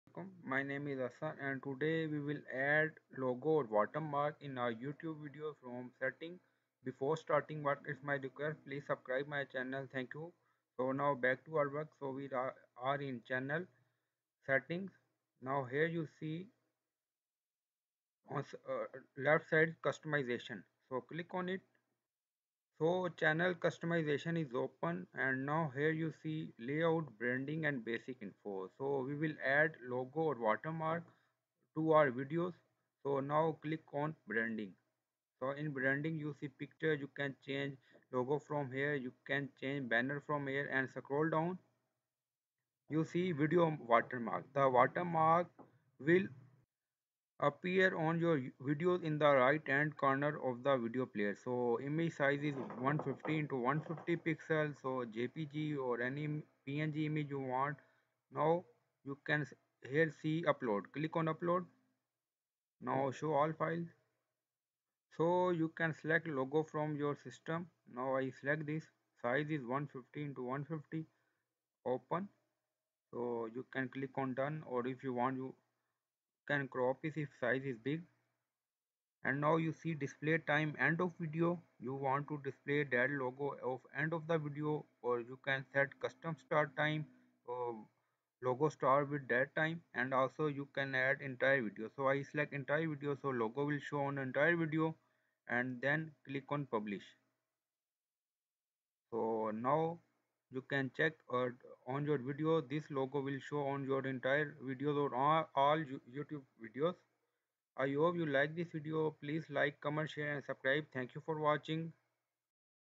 How to Add a Logo or Watermark to Your YouTube Videos | Watermark Branding
In this tutorial you will learn how to add watermark to brand youtube video.